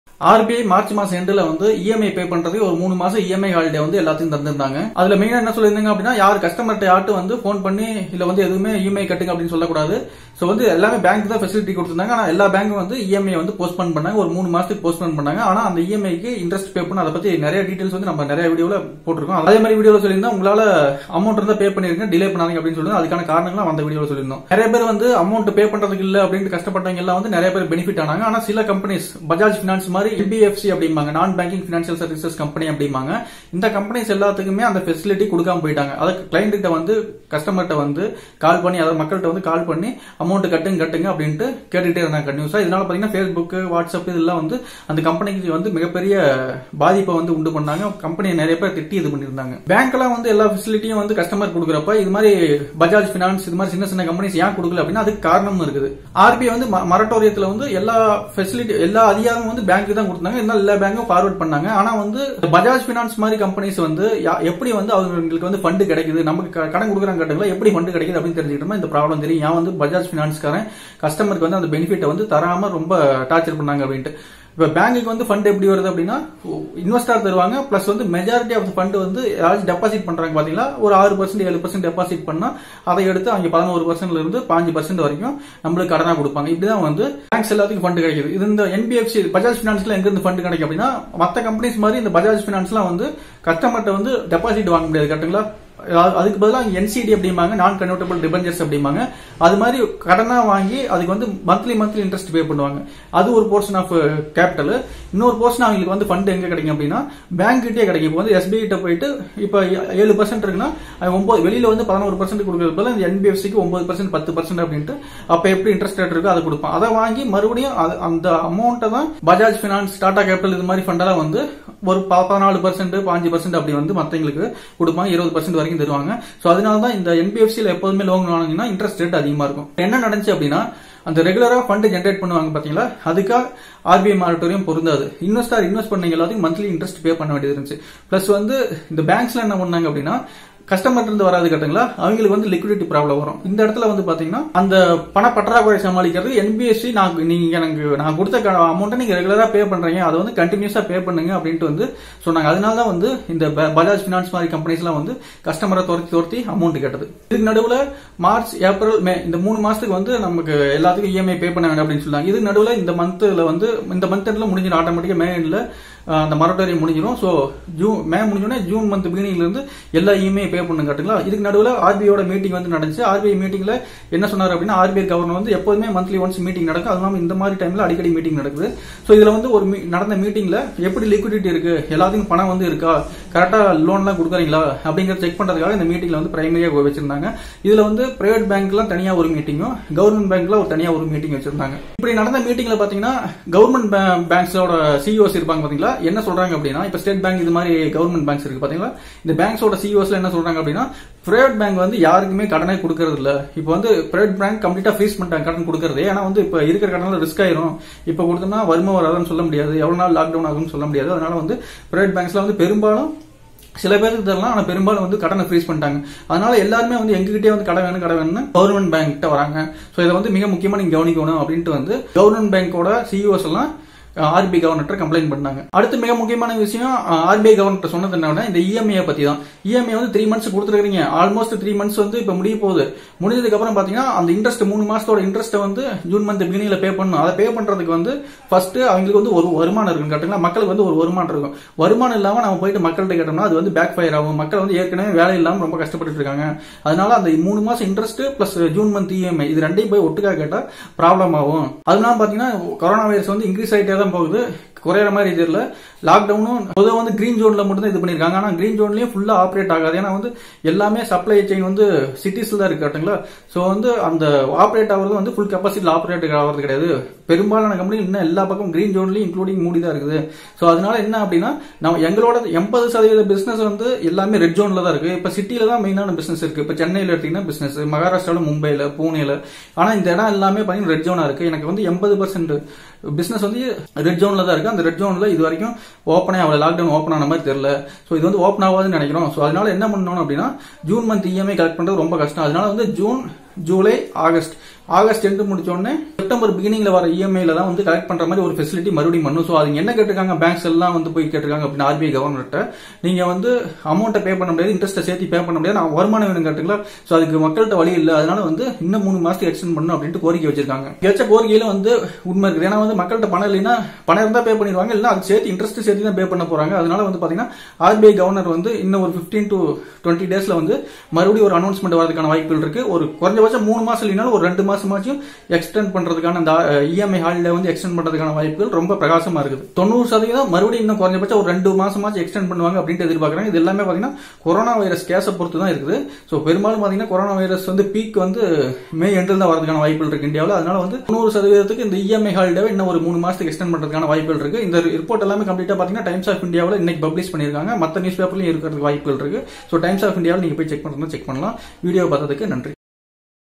ஆகஸ்ட் வரை EMI கட்ட வேண்டாமா ? EMI கட்டணுமா? | Bajaj Finance | HDFC | RBI | Explained
In this video we are explaining about the stock market in tamil we are covering the topics related to share tamil and queries related to money pechu in this video we explianed about the EMI payments for all knids of loans taken and the recent announcement by the RBI regading the loans and EMI repayment by customers

Also explained about the bajaj finance EMI and the issues in the EMI Payments. பஜாஜ் பைனான்ஸ் customers faced lot of problems in therir EMI payments.